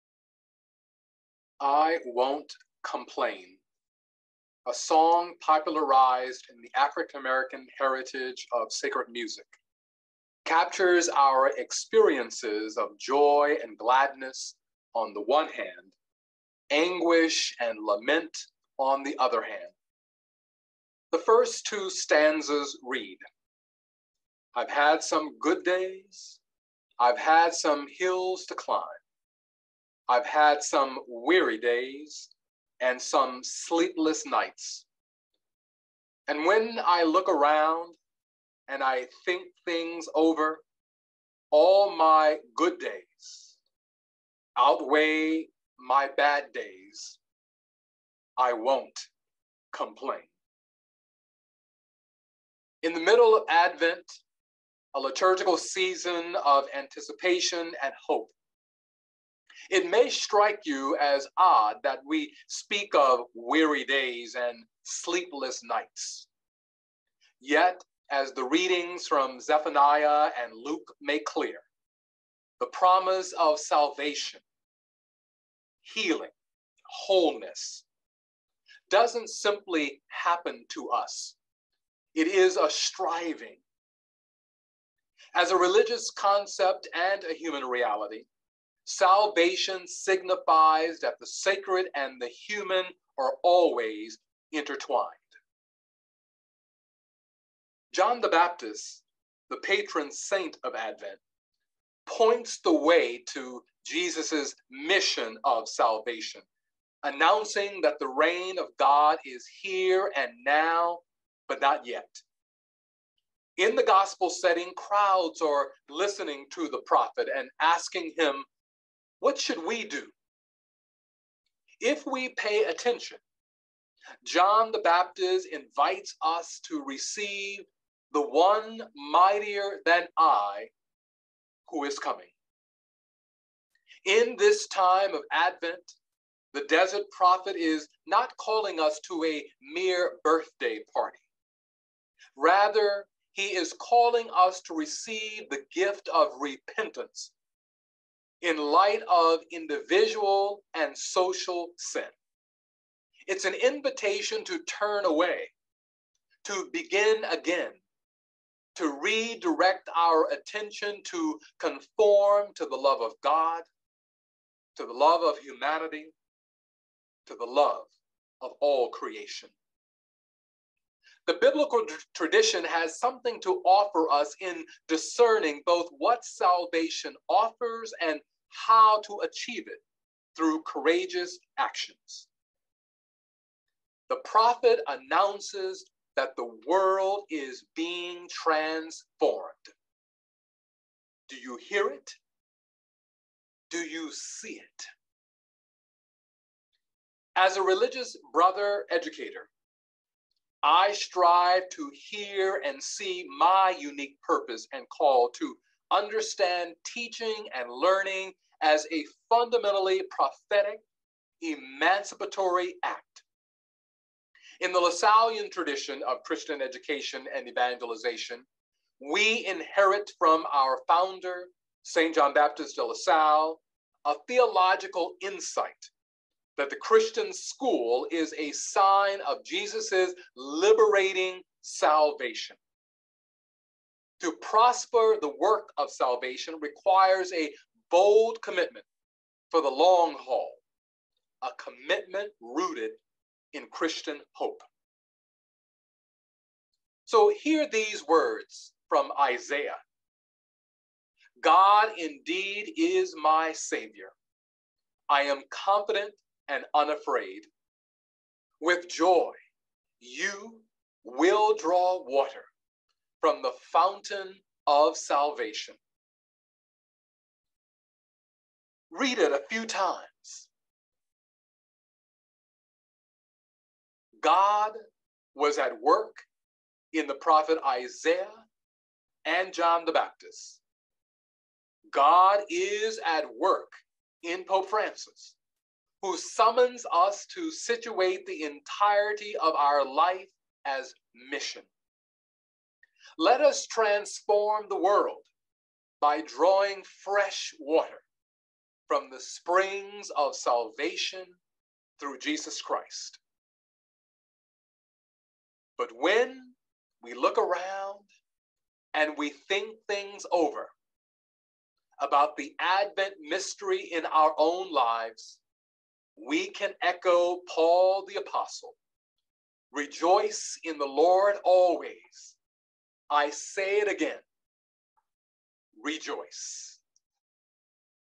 Sunday Reflection: Br. Ernest J. Miller, F.S.C. on the third Sunday of Advent
Lasallian Brother Ernest J. Miller reflects on the Sunday Mass readings for December 12, 2021, the third Sunday of Advent (Year C).